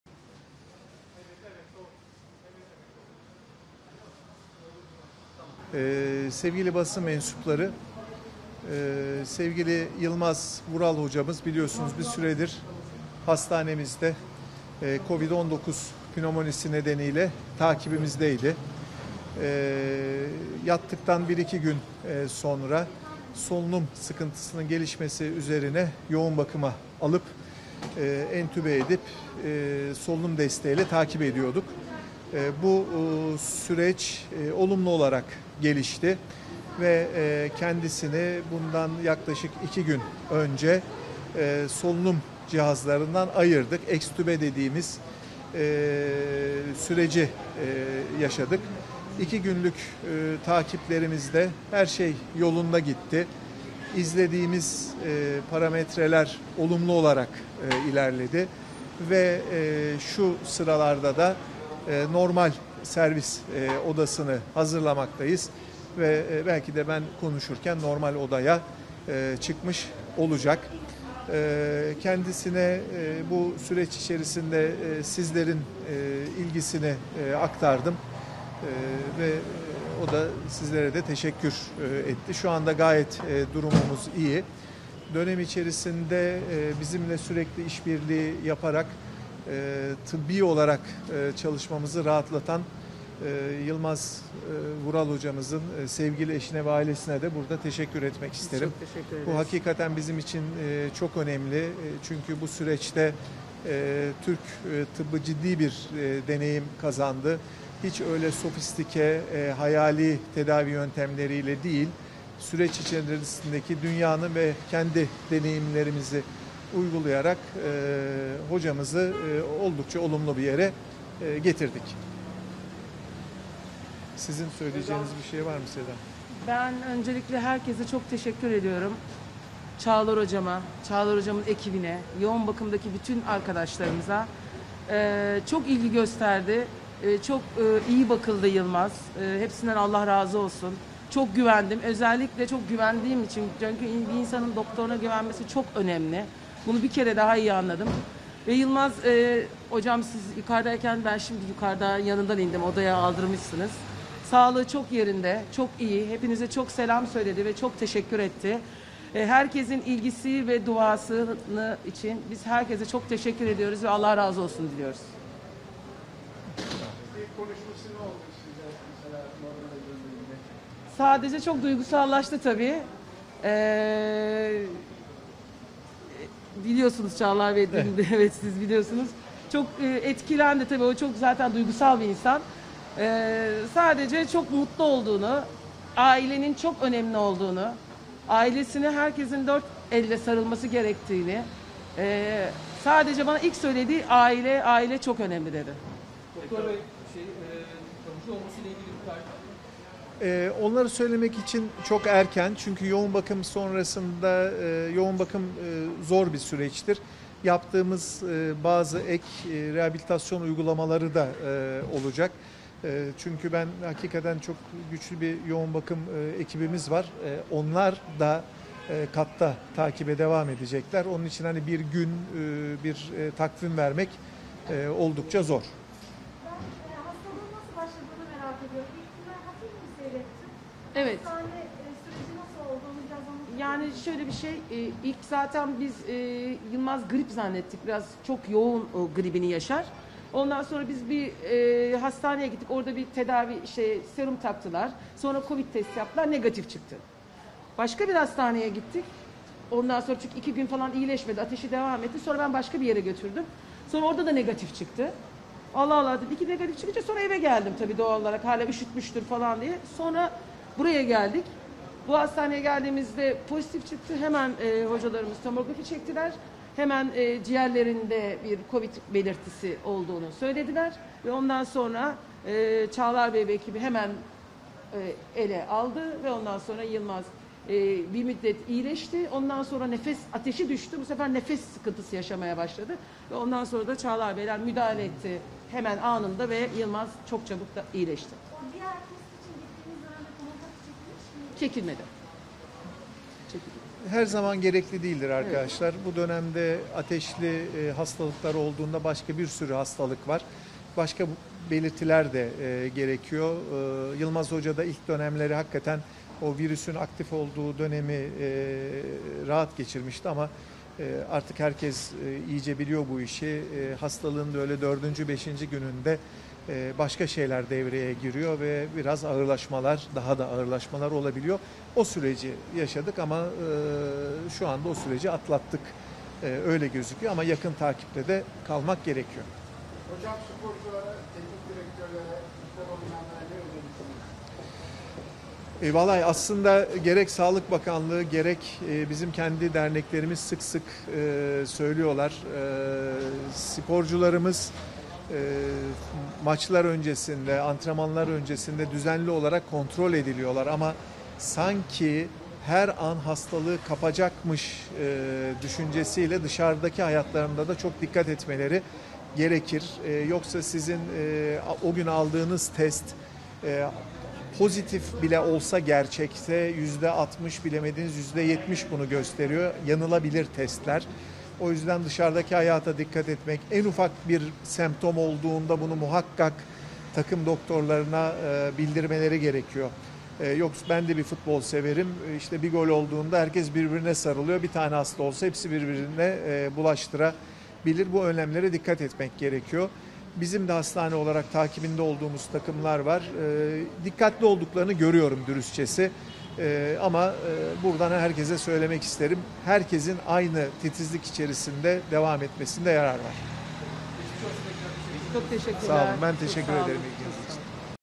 Yoğun bakımda tedavisi devam eden Yılmaz Vural hakkında resmi açıklama!
Korona virüs nedeniyle yoğun bakımda tedavi altına alınan Teknik Direktör Yılmaz Vural’ın yoğun bakım sürecini atlattığı ve normal odaya alındığı açıklandı.
Deneyimli teknik direktör Yılmaz Vural bir süredir korona virüs nedeniyle hastanede yoğun bakım ünitesinde tedavi görüyordu. Acıbadem Altunizade Hastanesi Başhekimi Prof. Dr. Çağlar Çuhadaroğlu ile Yılmaz Vural’ın eşi Seda Vural, başarılı teknik adamın sağlık durumu hakkında açıklama yaptı. Prof. Dr. Çuhadaroğlu, Vural’ın yoğun bakımda entübe edilerek tedavi gördüğünü ve iki gün önce solunum cihazlarından ayrıldığını belirtti. Çuhadaroğlu, Vural’ın normal odaya alındığı duyurdu.

“İki gün önce solunum cihazlarından ayırdık”
Acıbadem Altunizade Hastanesi Başhekimi Prof. Dr. Çağlar Çuhadaroğlu, “Yılmaz Vural Hocamız biliyorsunuz bir süredir hastanemizde Kovid-19 nedeniyle takibimizdeydi. Yattıktan 1-2 gün sonra solunum sıkıntısının gelişmesi üzerine yoğun bakıma alıp entübe edip, solunum desteğiyle takip ediyorduk. Bu süreç olumlu olarak gelişti ve kendisini bundan yaklaşık iki gün önce solunum cihazlarından ayırdık. Ekstübe dediğimiz süreci yaşadık. İki günlük takiplerimizde her şey yolunda gitti. İzlediğimiz parametreler olumlu olarak ilerledi ve şu sıralarda da normal servis odasını hazırlamaktayız. Belki de ben konuşurken normal odaya çıkmış olacak. Kendisine bu süreç içerisinde sizlerin ilgisini aktardım. Sizlere de teşekkür etti. Şu anda gayet durumumuz iyi. Bu süreçte Türk tıbbı ciddi bir deneyim kazandı. Hiç öyle sofistike, hayali tedavi yöntemleriyle değil süreç içerisindeki dünyanın ve kendi deneyimlerimizi uygulayarak hocamızı oldukça olumlu bir yere getirdik” dedi.
Yılmaz Vural’ın eşi Seda Vural ise, “Ben herkese çok teşekkür ediyorum. Özellikle çok güvendiğim için, bir insanın doktoruna güvenmesi çok önemli. Bunu bir kere daha iyi anladım. Ben şimdi yukarıda yanından indim, odaya aldırmışsınız. Sağlığı çok yerinde, çok iyi. Çok selam söyledi ve çok teşekkür etti. Herkesin ilgisi ve duası için biz herkese çok teşekkür ediyoruz ve Allah razı olsun diyoruz” diye konuştu.
Seda Vural, eşi Yılmaz Vural’ın kendine geldiği anlar ile ilgili, “Çok duygusallaştı tabii. Çok etkilendi, o çok duygusal bir insan zaten. Sadece çok mutlu olduğunu, ailenin çok önemli olduğunu, herkesin ailesine dört elle sarılması gerektiğini söyledi. Bana ilk söylediği aile, aile çok önemli dedi” şeklinde konuştu.

“Bir gün vermek oldukça zor”
Vural’ın ne zaman taburcu olacağı ile ilgili konuşan Prof. Dr. Çağlar Çuhadaroğlu, “Onları söylemek için erken. Yoğun bakım zor bir süreçtir. Yaptığımız bazı ek rehabilitasyon uygulamaları da olacak. Hakikaten çok güçlü bir yoğun bakım ekibimiz var. Onlar katta takibe devam edecekler. Onun için bir gün, bir takvim vermek oldukça zor” şeklinde konuştu.
Yılmaz Vural’ın hastalığının başlangıcı hakkında konuşan Seda Vural, “Biz, Yılmaz grip zannettik. Gribi çok yoğun yaşar. Ondan sonra biz hastaneye gittik orada serum taktılar. Sonra Kovid testi yaptılar, negatif çıktı. Başka bir hastaneye gittik, çünkü iki gün kadar iyileşmedi, ateşi devam etti. Sonra ben başka bir yere götürdüm. Orada da sonucu negatif çıktı. İki negatif çıkınca sonra eve geldik tabii doğal olarak. Sonra buraya geldik. Bu hastaneye geldiğimizde pozitif çıktı. Hemen hocalarımız tomografi çektiler. Hemen ciğerlerinde bir Kovid belirtisi olduğunu söylediler. Ve ondan sonra Çağlar Bey ve ekibi hemen ele aldı ve ondan sonra Yılmaz bir müddet sonra iyileşti. Ateşi düştü ama bu sefer nefes sıkıntısı yaşamaya başladı. Ondan sonra müdahale ettiler ve Yılmaz çok çabuk iyileşti” dedi.
Prof. Dr. Çağlar Çuhadaroğlu konuşmasının devamında, “Yılmaz Hoca ilk dönemleri, virüsün aktif olduğu dönemi rahat geçirmişti ama artık herkes iyice biliyor bu işi, hastalığın dördüncü beşinci gününde başka şeyler devreye giriyor ve daha da ağırlaşmalar olabiliyor. O süreci yaşadık ama şu anda o süreci atlattık. Öyle gözüküyor ama yakın takipte de kalmak gerekiyor” ifadelerini kullandı.
Sporculara korona virüs hakkında tavsiyelerde bulunan Prof. Dr. Çağlar Çuhadaroğlu, “Gerek Sağlık Bakanlığı, gerek bizim kendi derneklerimiz sık sık söylüyorlar, sporcularımız maçlar öncesinde, antrenmanlar öncesinde düzenli olarak kontrol ediliyor ama sanki her an hastalığı kapacakmış düşüncesiyle dışarıdaki hayatlarında da çok dikkat etmeleri gerekir. Yoksa sizin o gün aldığınız test negatif bile olsa gerçekte yüzde altmış, bilemediniz yüzde yetmiş bunu gösteriyor. Yanılabilir testler. O yüzden dışarıdaki hayata dikkat etmek en ufak bir semptom olduğunda bunu muhakkak takım doktorlarına bildirmeleri gerekiyor” açıklamasını yaptı.